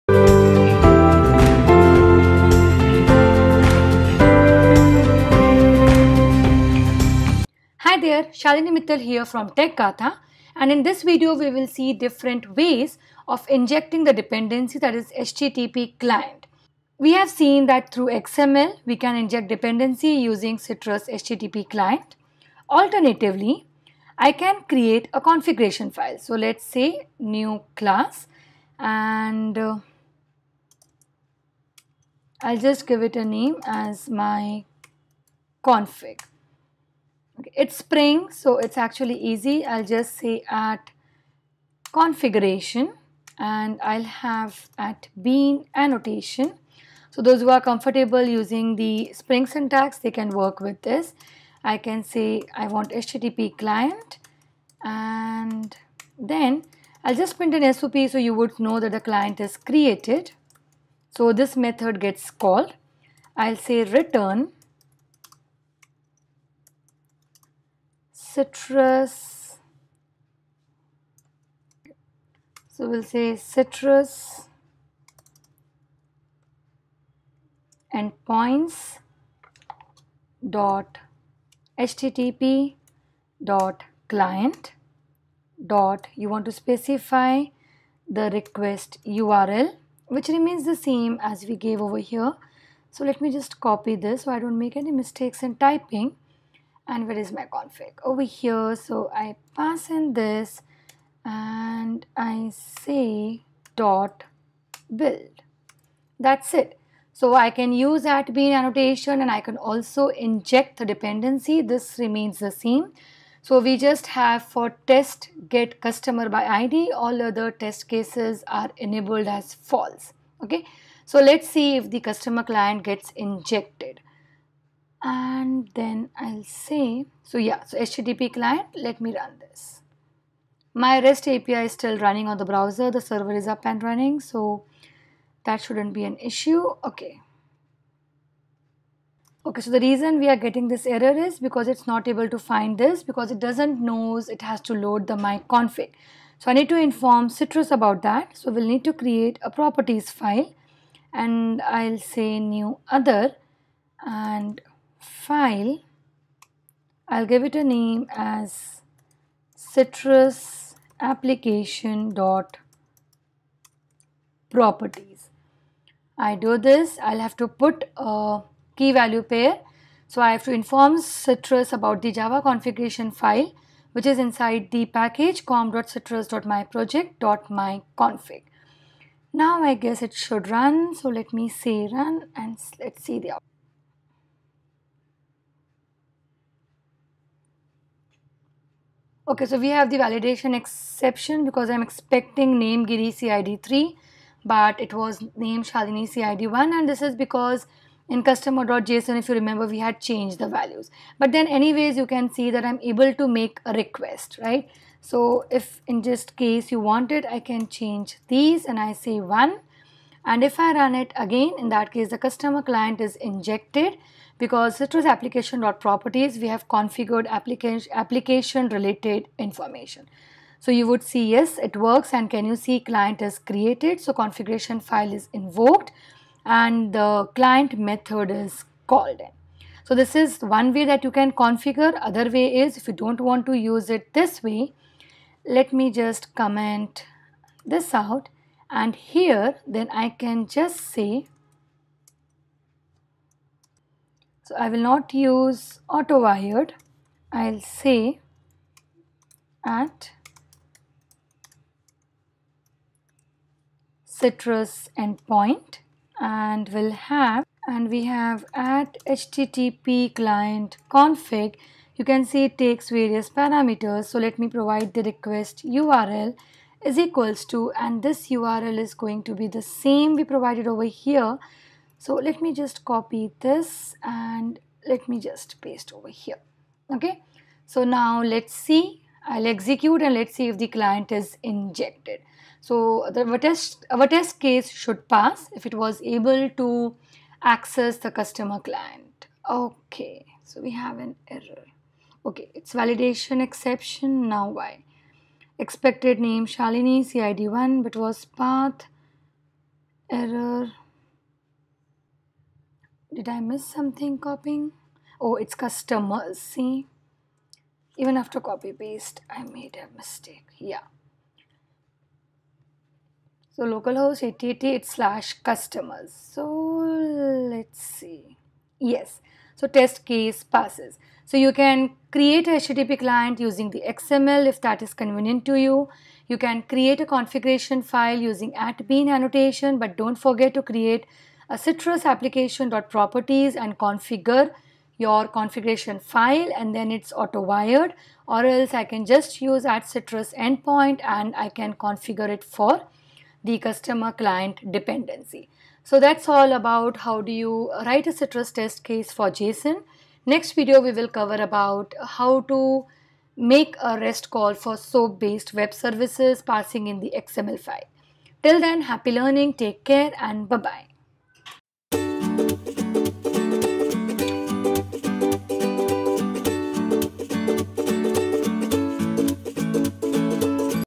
Citrus Integration Testing Framework 12 - HttpClient Configuration ways
This video covers how you can inject the HttpClient dependency in different ways required for making REST API calls